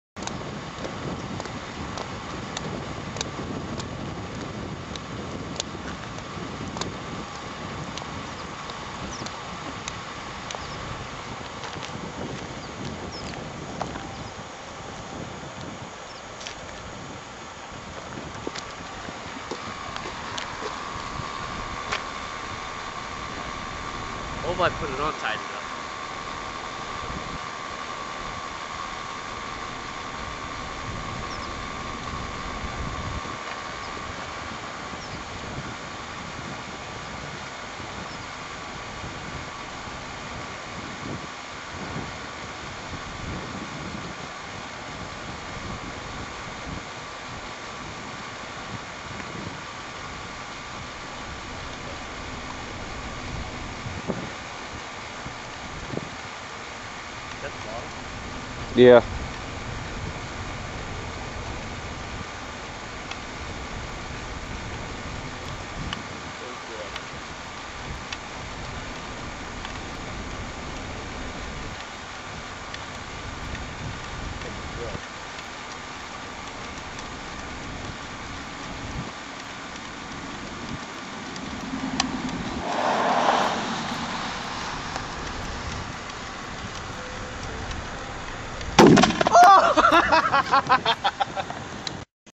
First Attempt at Dry Ice Bomb
After studying videos and reading on this experiment, i figured i would give it a try. Had to upload this one because it was my first interaction with Sublimation and its outcome. More videos to come, these things are addicting to do. My laugh is priceless!